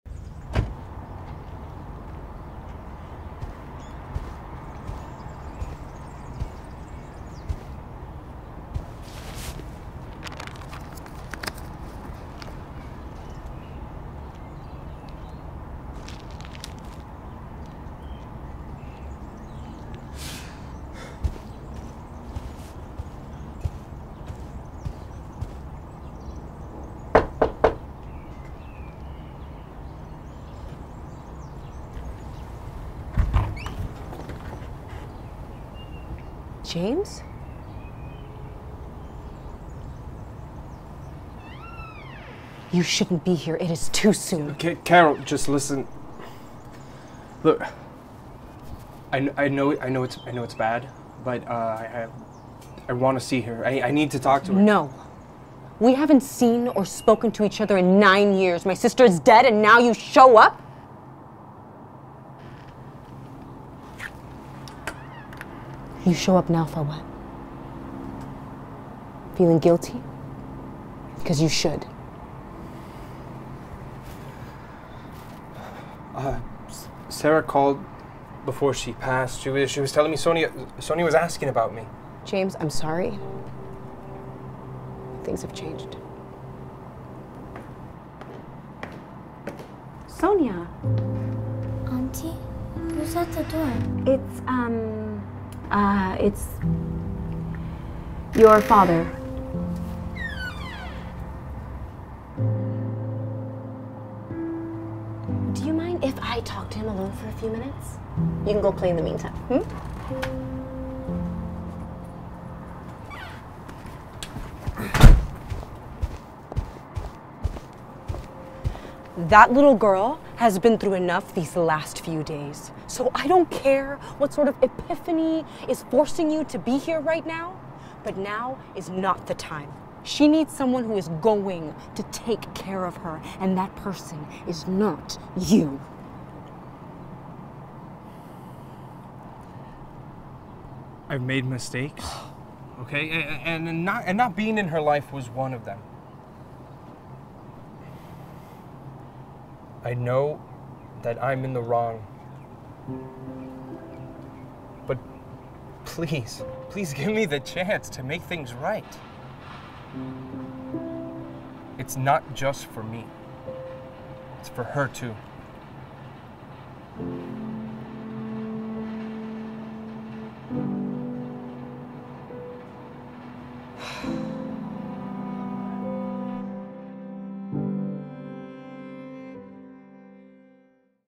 New Life, Hard Choice | Proof Of Concept
James, a remorseful man, must make his way back into his daughter’s life after walking out on her nearly a decade ago, before he loses the chance to amend his mistakes.

Starring...
Roger Torres as James Romero
Dayana Corton as Carol Miller
Angelica Cinti as Sonia Romero

Producer(s): Samuel Vargas & Leda Berhama
Director & Writer: Marcus Limbo
Co-Writer: Kevin Martinez
UPM: Alan Bendahan
1st AD: Alexa Gonzalez
Script Supervisor: Kevin Martinez

Director of Photography: Brian Frias
1st AC: Eric Avila
Gaffer: Alejandro Lopez
Grip: Havana Durango & Alan Bendahan

Production Designer: Joel Guevara
Production Design Assistant: Steven Almendares

Sound Mixer & Boom Operator: Christian Tejera

Makeup Artist & Hair Stylist: Paxton Lee

Production Assistant: Andy Calzadilla

Editor &  Sound Designer: Hanssel Guerra
Colorist: Brian Frias